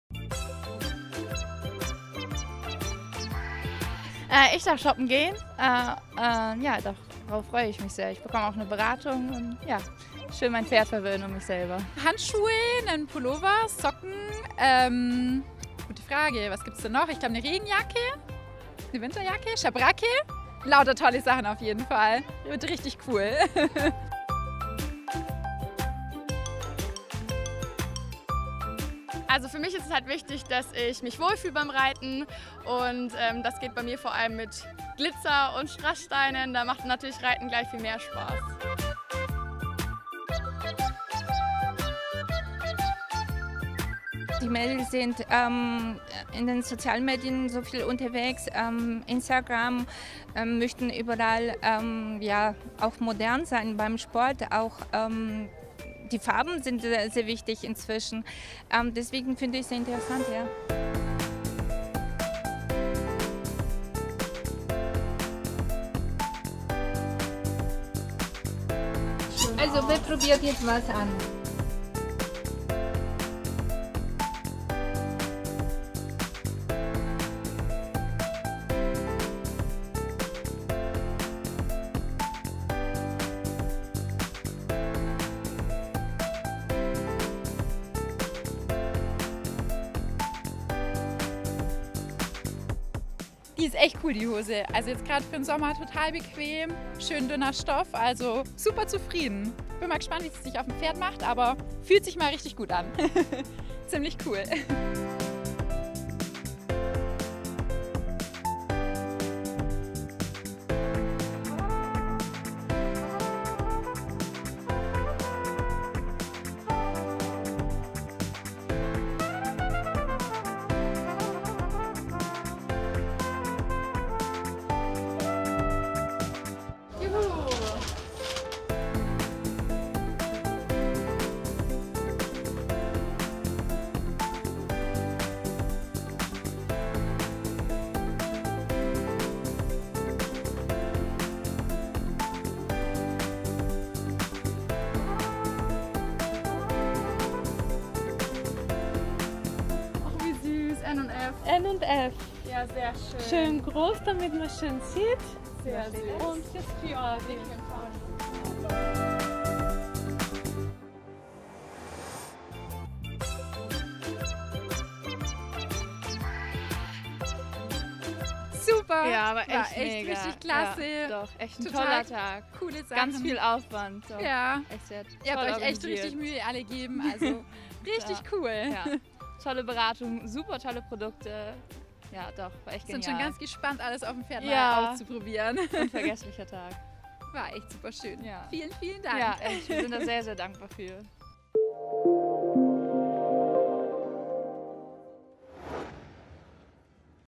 On the road with @fjordifaunita | Shopping - Aktion | Pferd international | München 2017
Shopping - Aktion bei der Pferd international! Die zwei Gewinnerinnen dürfen sich mit ihren Shopping-Beraterinnen verwöhnen lassen. Seht selbst..! Den kostenlosen Livestream der Pferd international in München findet ihr bei uns auf der Seite